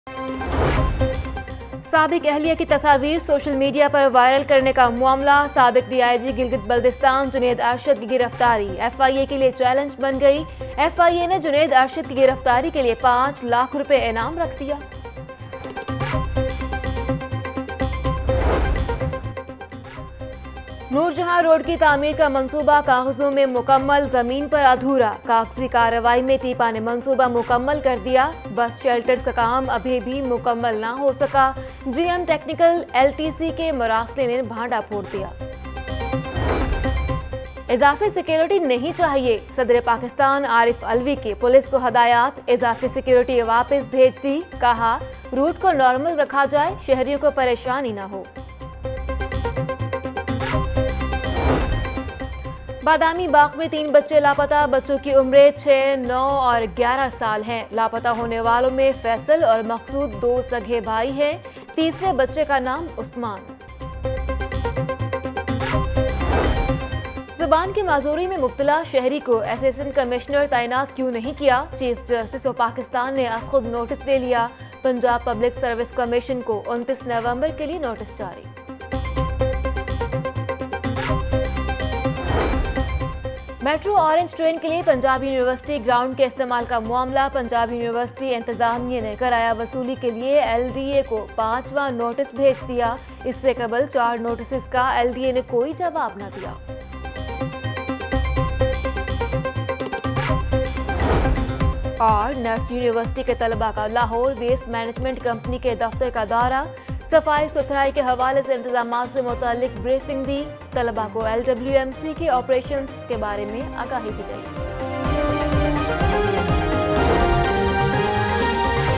11 PM Headlines Lahore News HD – 17th November 2018
Sharif family meeting with Shehbaz Sharif at NAB office. “No helmet no fuel” for Lahore bikers. Fayyaz ul Hassan breaks Punjab Film censor board